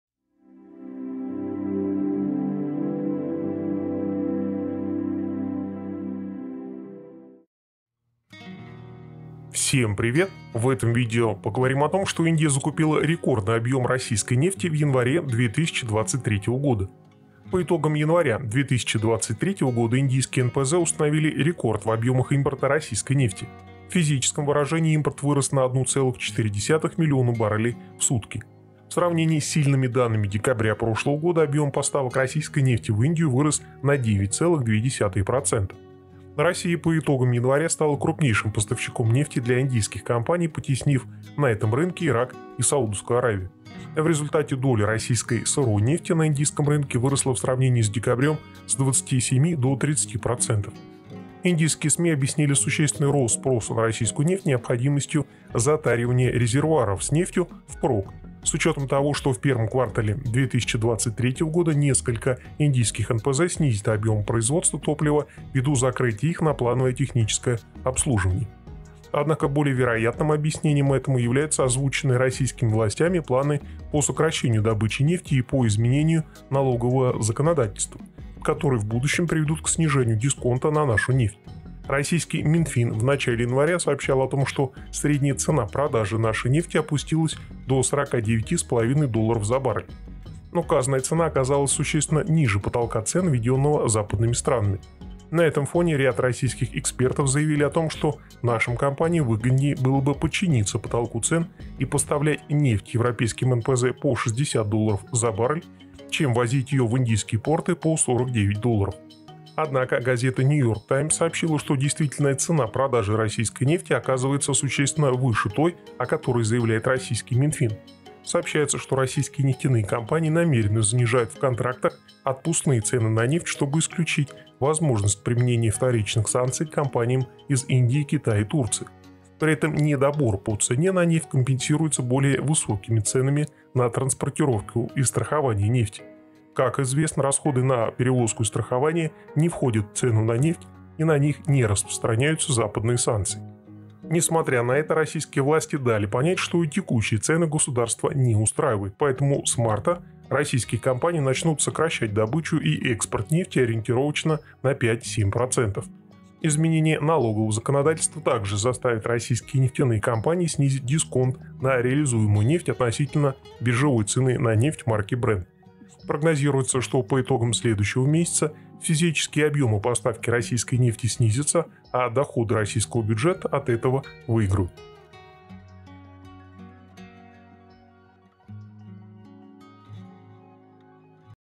Индия закупила рекордный объем российской нефти в январе 2023 года
Индия закупила рекордный объем российской нефти в январе 2023 года. Российские компании увеличили объем поставки нефти в Индию.  Российские компании поставляют нефть по более высокой цене, чем указано в контракте. Россия начнет сокращать добычу нефти с марта 2023 года